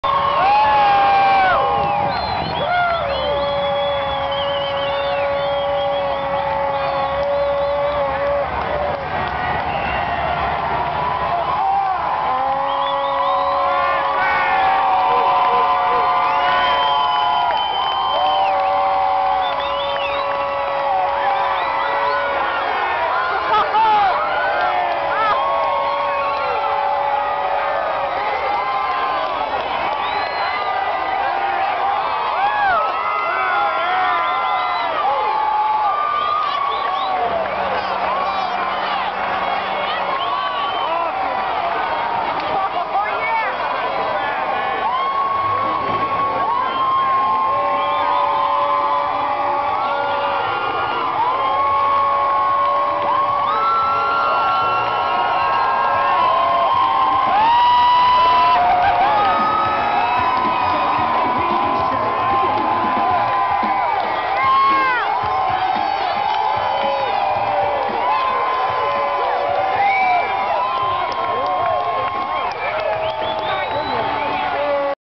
Phillies Parade 1
Featuring Pat Burrell, Harry Kalas, the Philly Phanatic